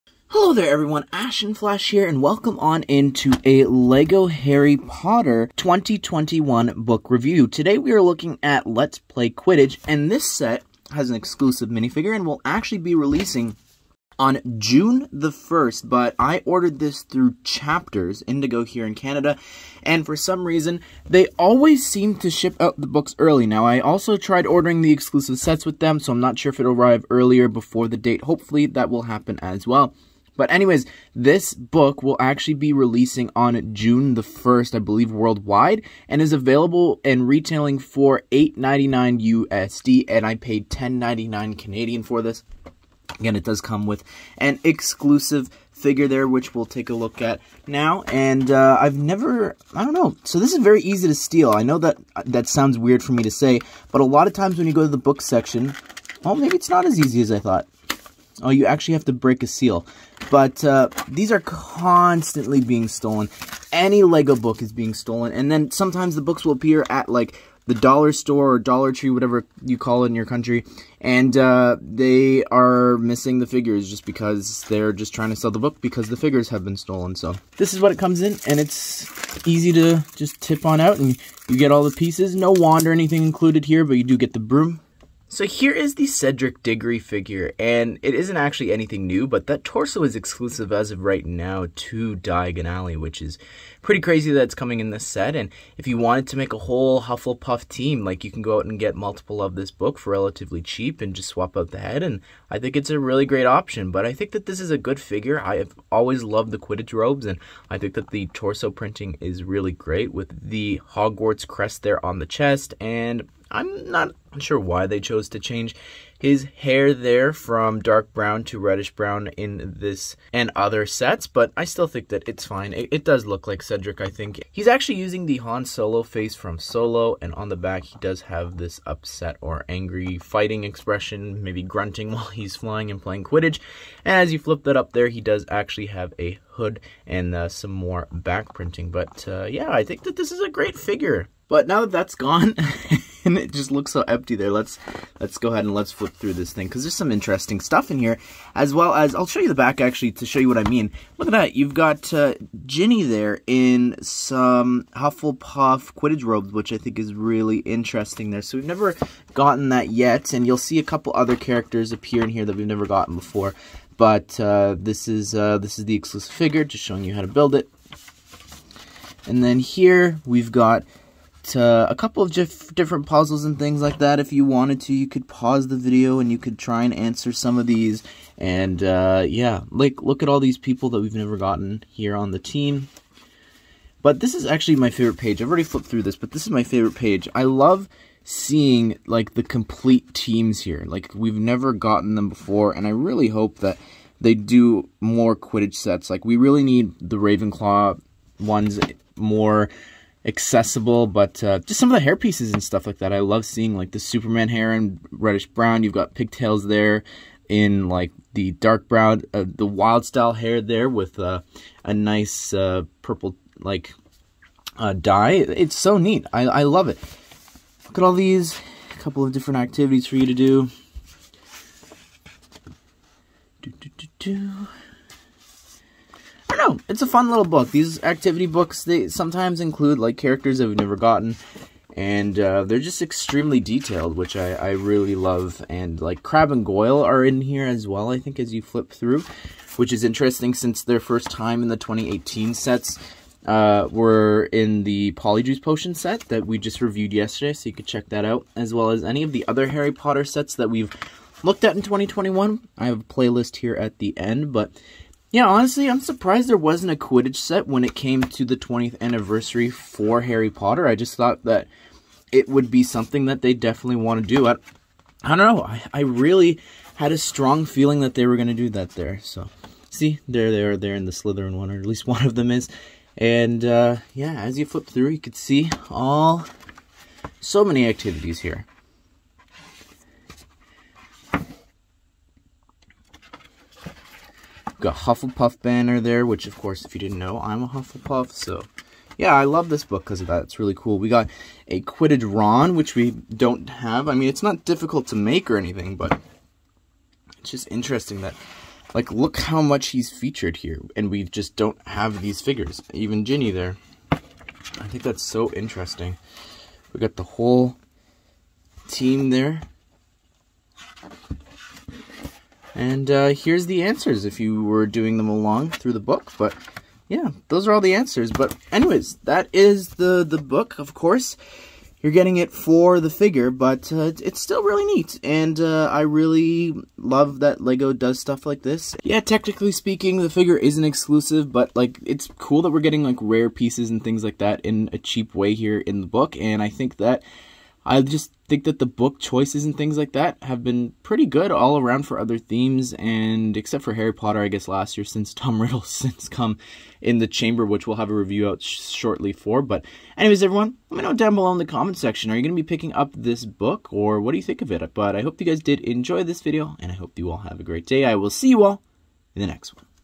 LEGO Harry Potter NEW Let's Play Quidditch Book Early Review - WITH Exclusive Cedric Diggory
Today, we're taking an early look at the new LEGO Harry Potter Let's Play Quidditch book that comes with an exclusive Cedric Diggory minifigure!